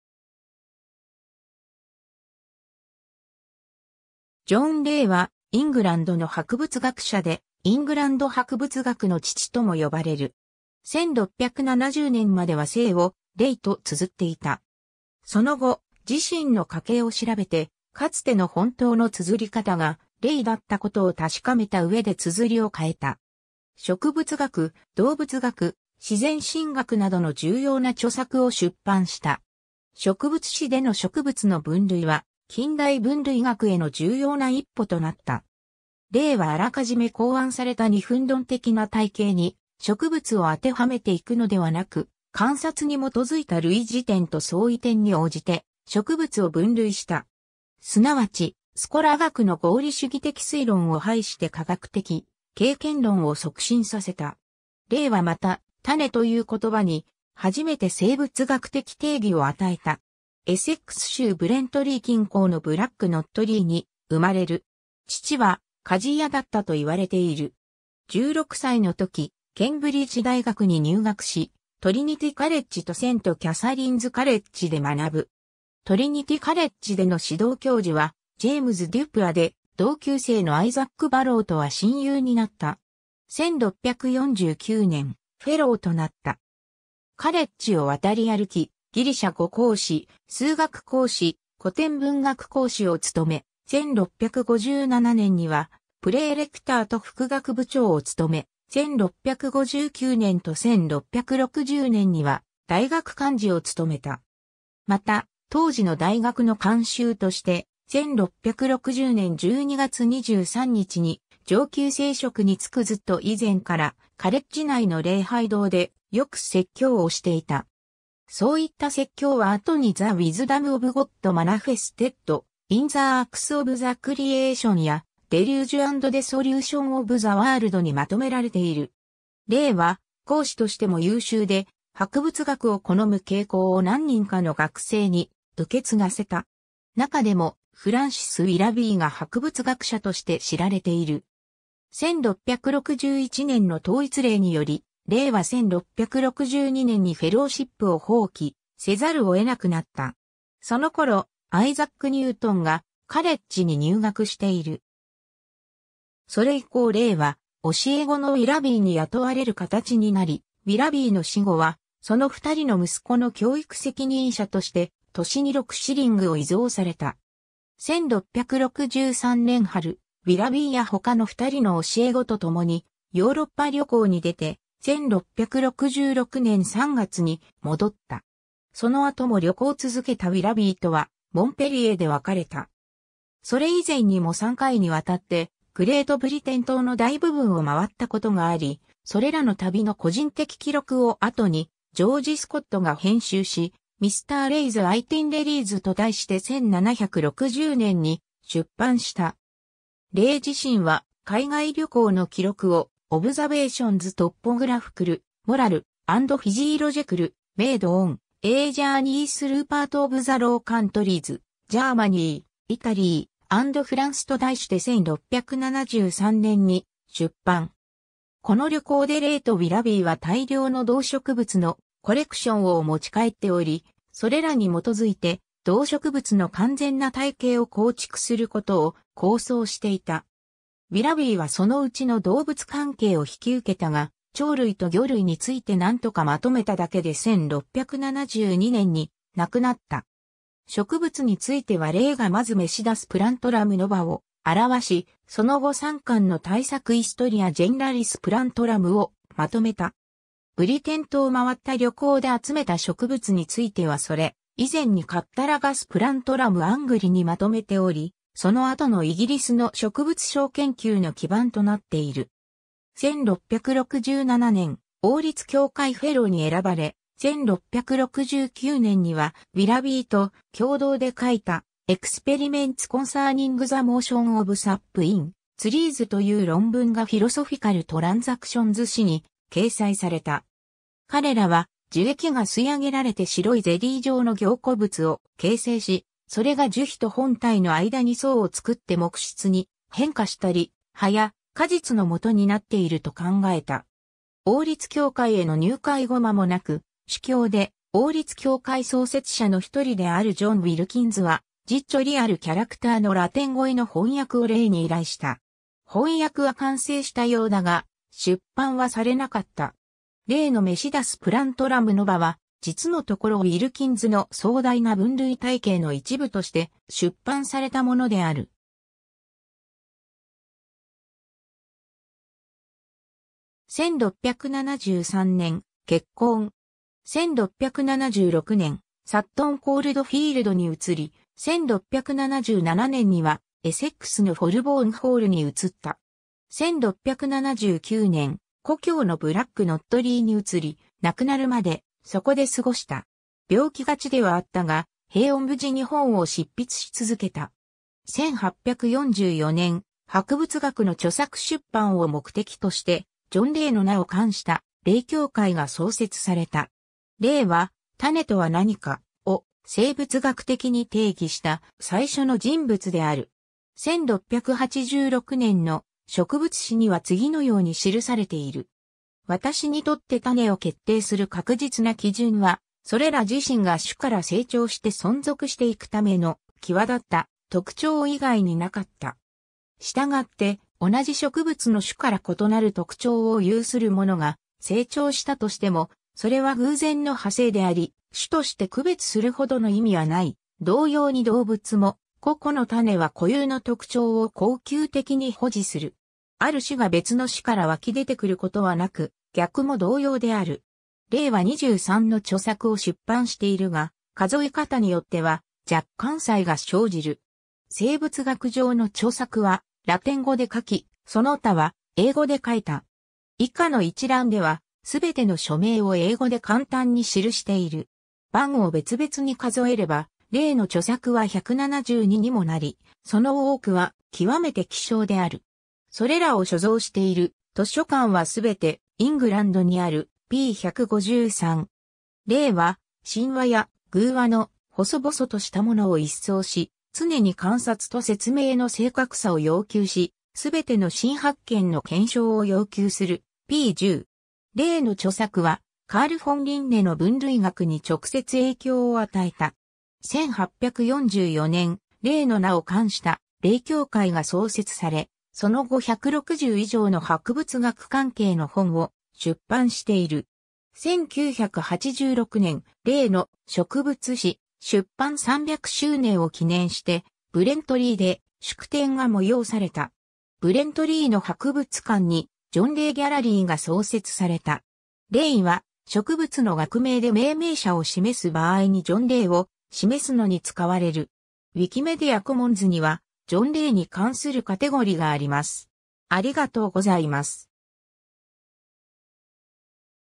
ジョン・レイ (博物学者)
ジョン・レイ（John Ray、1627年11月29日 - 1705年1月17日）は、イングランドの博物学者で、「イングランド博物学の父」とも呼ばれる。
1670年までは姓を "Wray" と綴っていた。
その後、自身の家系を調べてかつての本当の綴り方が "Ray" だったことを確かめた上で綴りを替えた。
 植物学、動物学、自然神学などの重要な著作を出版した。
『植物誌』(Historia Plantarum)での植物の分類は近代分類学への重要な一歩となった。
レイは予め考案された二分論的な体系に植物を当てはめていくのではなく、観察に基づいた類似点と相違点に応じて植物を分類した。
すなわち、スコラ学の合理主義的推論を排して科学的経験論を促進させた。
レイはまた、「種 (species)」という言葉に初めて生物学的定義を与えた。
 エセックス州ブレントリー近郊のブラック・ノットリーに生まれる。
父は鍛冶屋だったと言われている。
16歳のときケンブリッジ大学に入学し、トリニティ・カレッジとセント・キャサリンズ・カレッジで学ぶ。
トリニティ・カレッジでの指導教授は James Duport で、同級生のアイザック・バローとは親友になった。
1649年、フェローとなった。
カレッジを渡り歩き、ギリシャ語講師 (1651)、数学講師 (1653)、古典文学講師 (1655) を務め、1657年には praelector と副学部長を務め、1659年と1660年には大学幹事を務めた。
また、当時の大学の慣習として、1660年12月23日に上級聖職に就くずっと以前からカレッジ内の礼拝堂でよく説教をしていた。
そういった説教は後に The wisdom of God manifested in the works of the creation や Deluge and Dissolution of the World にまとめられている。
レイは講師としても優秀で、博物学を好む傾向を何人かの学生に受け継がせた。
中でもフランシス・ウィラビーが博物学者として知られている。
 1661年の統一令により、レイは1662年にフェローシップを放棄せざるを得なくなった。
そのころ、アイザック・ニュートンがカレッジに入学している。
それ以降レイは教え子のウィラビーに雇われる形になり、ウィラビーの死後はその2人の息子の教育責任者として年に6シリングを遺贈された。
 1663年春、ウィラビーや他の2人の教え子と共にヨーロッパ旅行に出て、1666年3月に戻った。
その後も旅行続けたウィラビーとはモンペリエで別れた。
それ以前にも3回（1658年、1661年、1662年）にわたってグレートブリテン島の大部分を回ったことがあり、それらの旅の個人的記録を後にジョージ・スコットが編集し Mr Ray's Itineraries と題して1760年に出版した。
レイ自身は海外旅行の記録を Observations topographical, moral, and physiological, made on a Journey through part of the Low Countries, Germany, Italy, and France と題して1673年に出版。
この旅行でレイとウィラビーは大量の動植物のコレクションを持ち帰っており、それらに基づいて動植物の完全な体系を構築することを構想していた。
ウィラビーはそのうちの動物関係を引き受けたが、鳥類と魚類について何とかまとめただけで1672年に亡くなった。
植物についてはレイがまず Methodus plantarum nova (1682) を著し、その後3巻の大作 Historia generalis plantarum (1686, 1688, 1704) をまとめた。
ブリテン島を回った旅行で集めた植物についてはそれ以前に Catalogus plantarum Angliae (1670) にまとめており、その後のイギリスの植物相研究の基盤となっている。
 1667年、王立協会フェローに選ばれ、1669年にはウィラビーと共同で書いた Experiments concerning the Motion o...